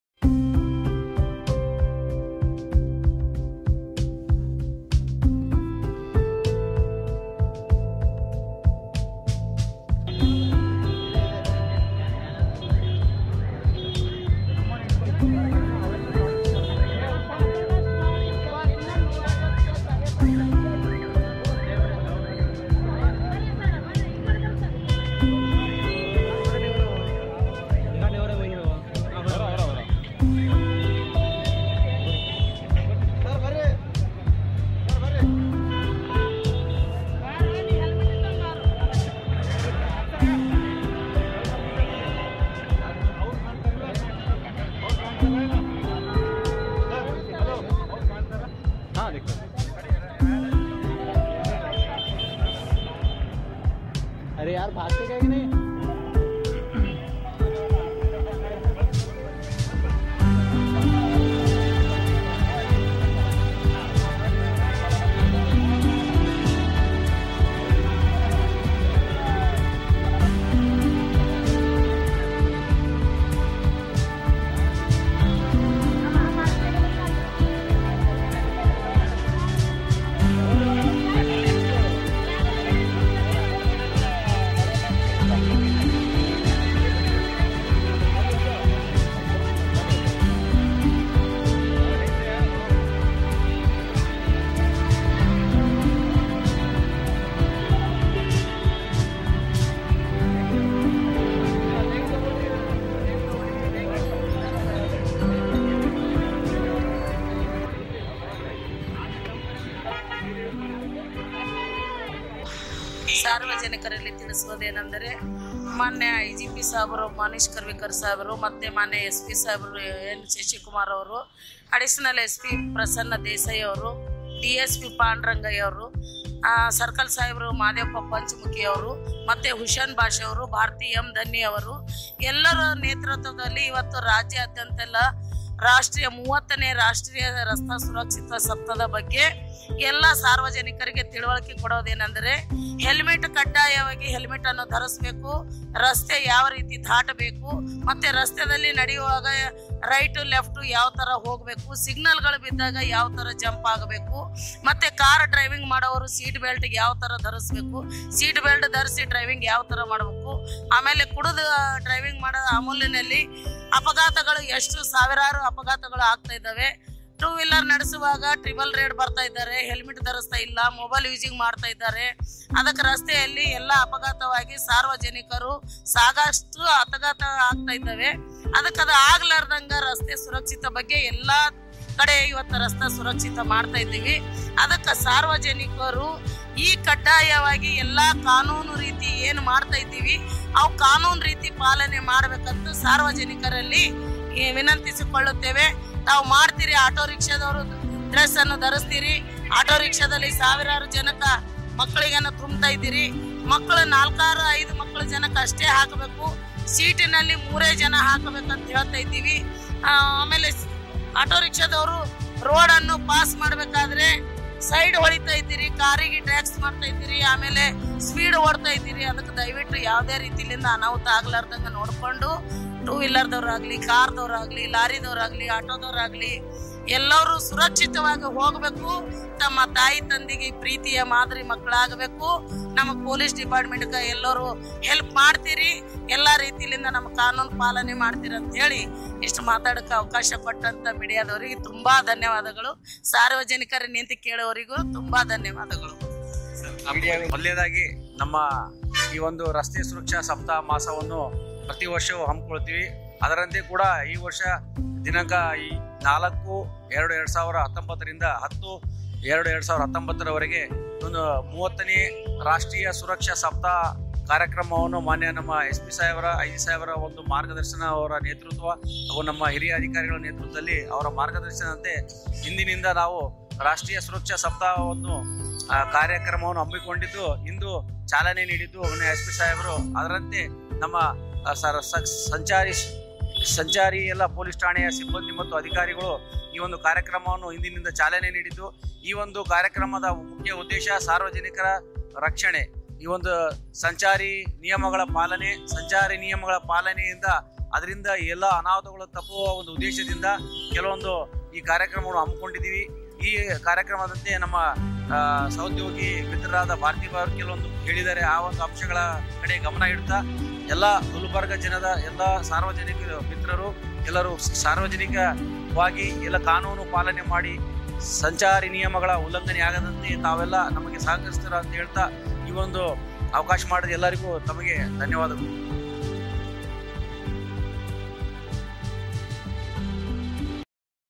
ROAD SAFETY WEEK awareness program by KALABURAGI TRAFFIC POLICE. - 042
(This Video is made by MyCityOnline)

As part of their ROAD SAFETY WEEK awareness program the TRAFFIC POLICE of KALABURAGI (GULBARGA) gave the Flowers as compliment and thanks to the Bike Riders and other commuters who were obeying the Traffic Rules (like not jumping the red signal, wearing the HELMET, not over speeding, etc.)

Definitely like this act of Traffic Police will change the mindset of people and not only encourage them to follow the traffic rules but also it will create good image of traffic police in public eyes.